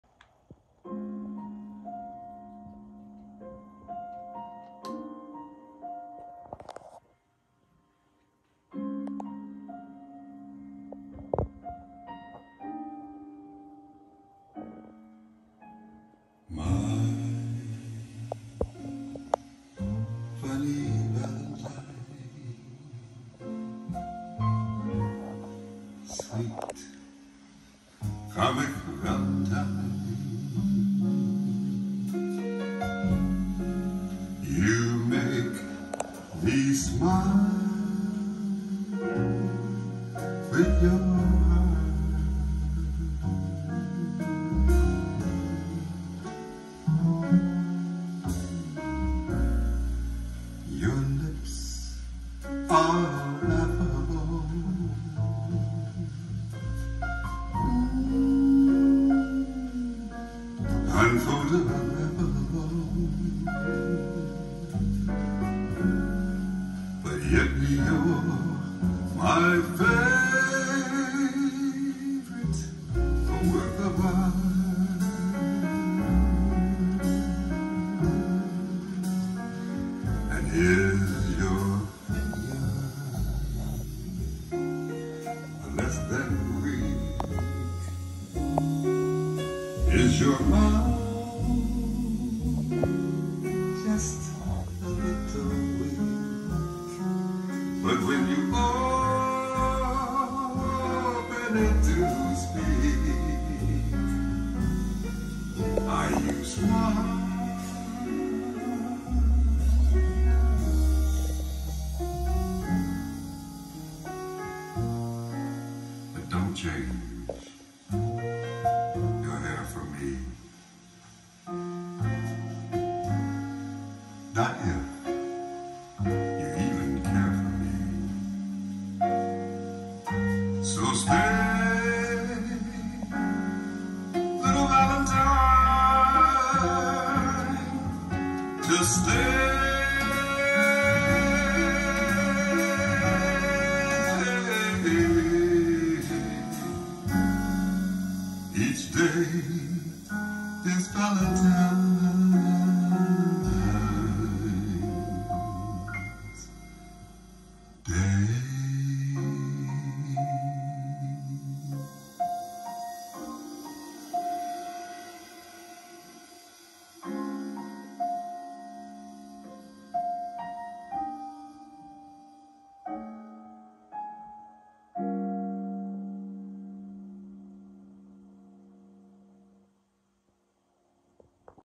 McIntosh MC-40 test 4 Metal Radiotron 6L6
McIntosh MC-40 metal Radiotron 6L6 Tannoy Canterbury GR . Check my other video same amps same sond 6P3S-E tube test